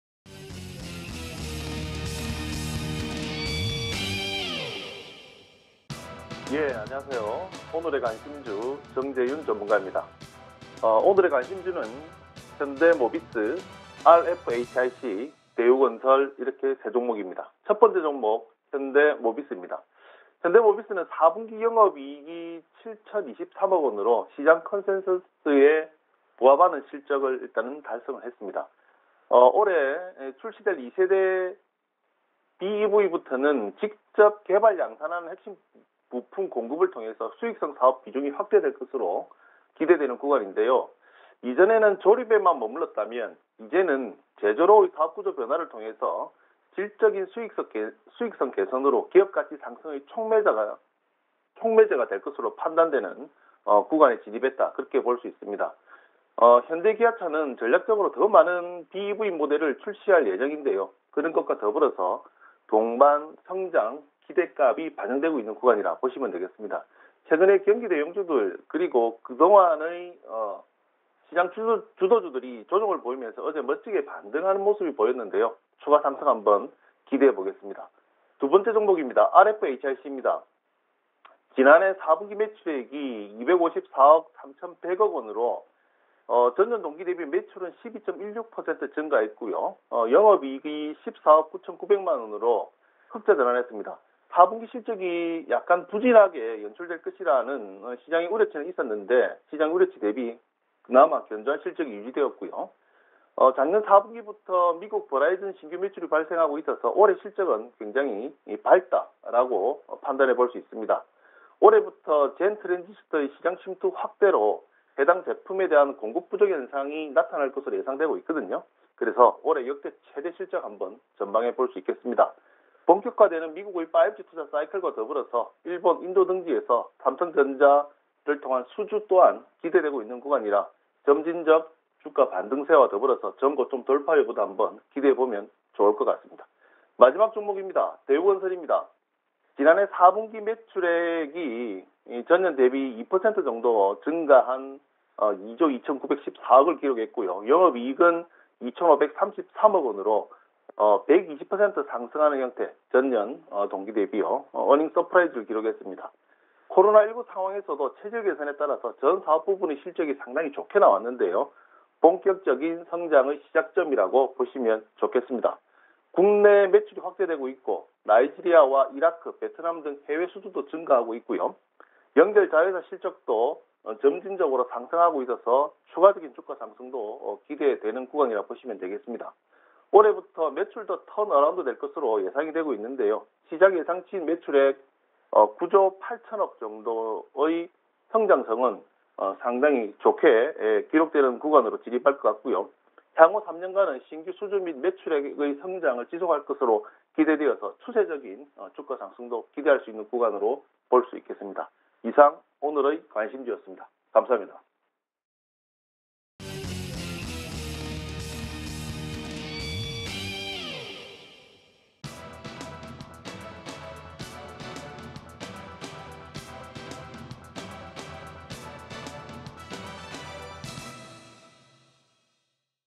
현대모비스, RFHIC, 대우건설(오늘의 관심주/21.02.02)
1.  현대모비스(012330)
 4분기 영업익 7,023억원으로 시장 컨센서스 6,673억원 부합
 올해 출시될 2세대 BEV부터 핵심부품 공급 통해 높은 수익성 사업 비중 확대
 조립 → 제조 사업구조 변화…질적 수익성 개선 및 기업가치 상승 촉매제될 것으로 판단
 현대·기아차는 전략적으로 더 많은 BEV모델 출시해 갈 예정…동반 성장 기대
 (출처, 메리츠증권)

2. RFHIC(218410)
 지난해 4분기 매출 254.3억(YoY+12.16%), 영업익 14.9억(흑자전환) 잠정 집계
 2020년 4분기부터 미국 버라이즌향 신규 매출 발생…올해 실적 전망 밝게 해줌
 올해 GaN 트랜지스터 시장 침투율 확대, 공급부족 현상 예상… 올해 역대 최고실적 전망
 본격화되는 미국 5G 투자사이클과 함께 일본, 인도 등에서 삼성전자 통한 수주 기대
 (출처, 리딩투자증권)

3. 대우건설(047040)
 지난해 4분기 매출 2조 2,914억원(YoY-1%), 영업익 2,533억원(+465%)…어닝 서프라이즈
 코로나19 상황에서도 체질 개선 따라 전사업부문 실적 개선, 본격적 성장의 시작점
 국내 매출확대 및 나이지리아와 이라크, 베트남 등 해외수주 증가, 연결 자회사 실적증가 기대
 2021년부터 매출도 턴어라운드 기대,  시장 예상치 매출액 9조 8,000억(YoY+20.4%)
 향후 3년간은 신규 수주 및 매출액 성장 지속
 (출처, 대신증권)